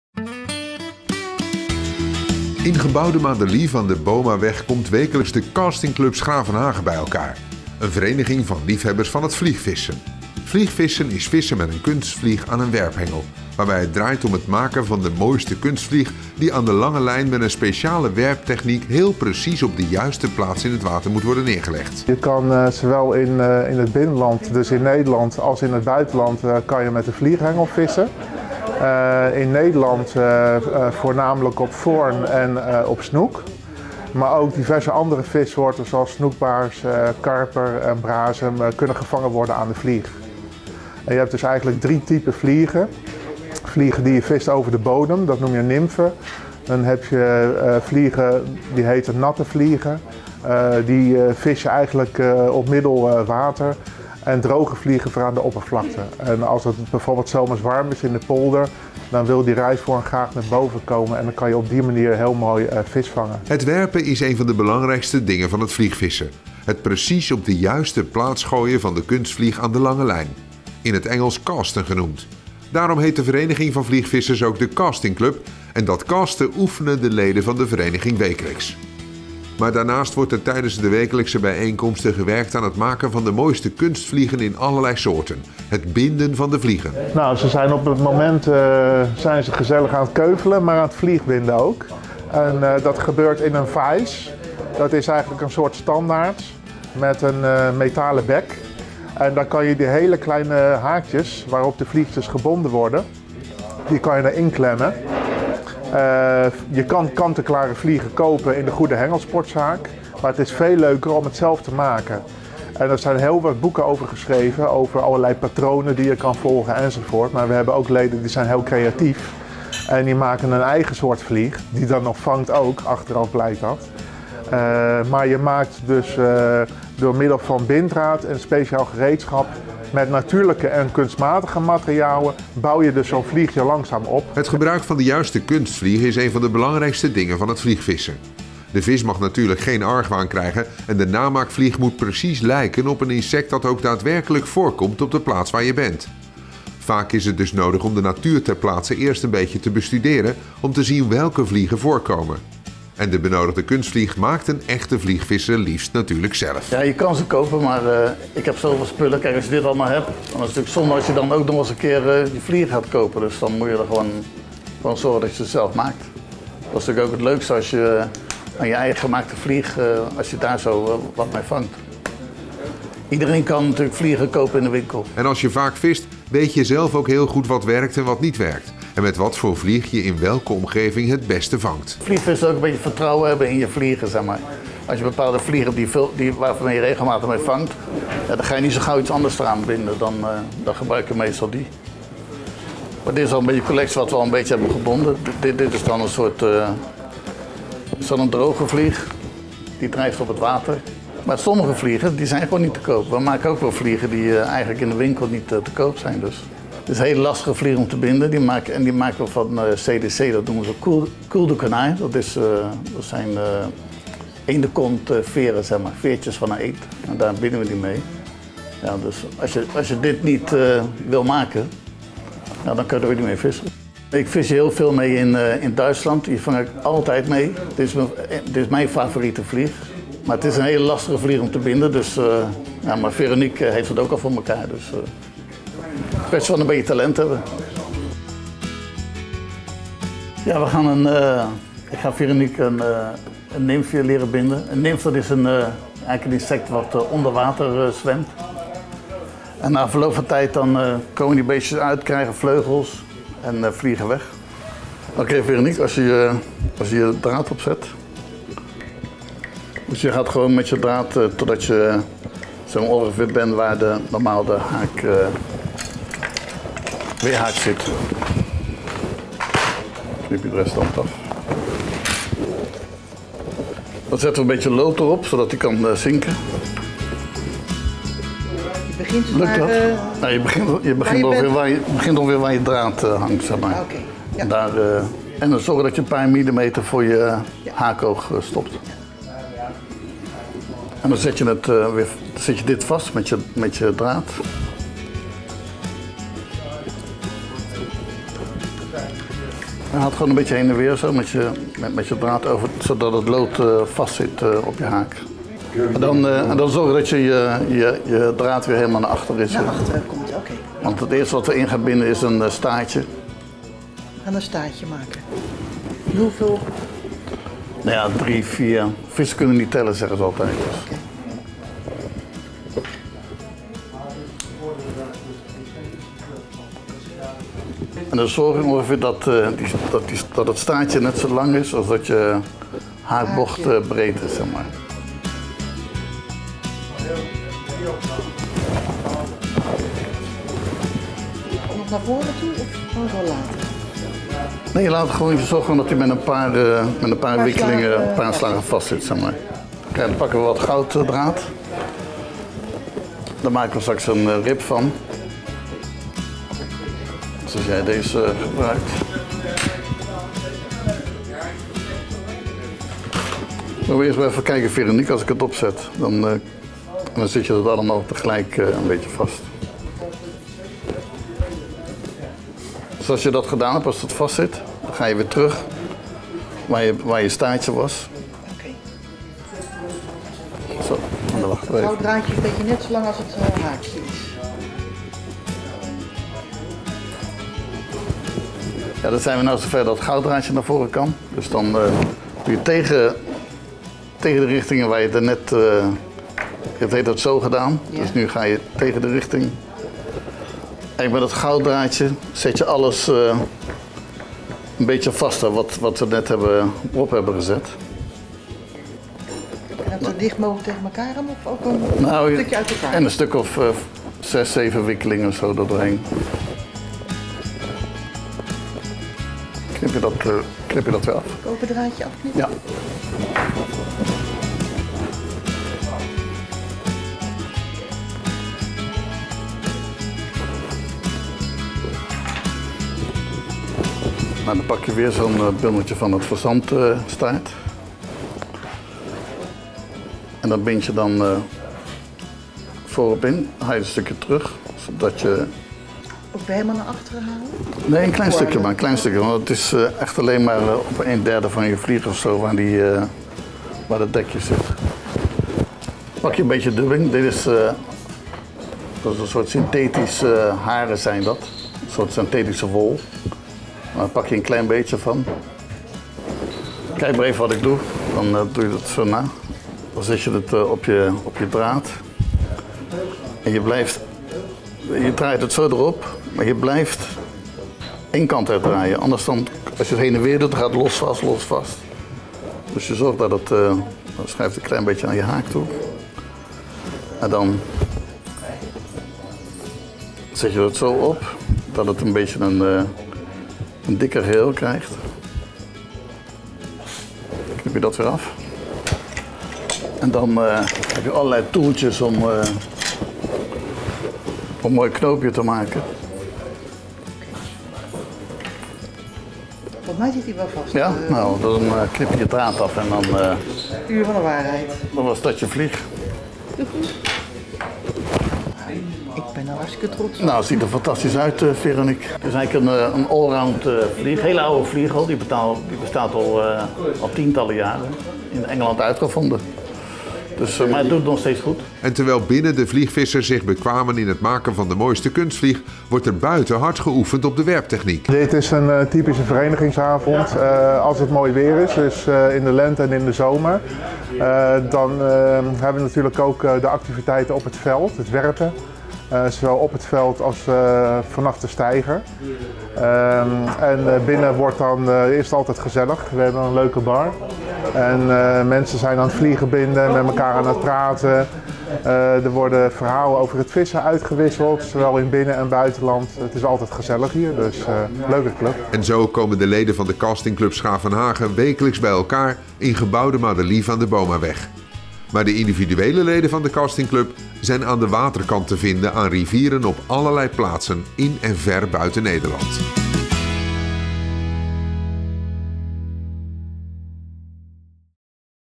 Castingclub 's Gravenhage Vliegen maken Deel 3
In deze aflevering van onze tv-serie staat het binden van een kunstvlieg centraal. In dit geval een nimf van Engelse origine.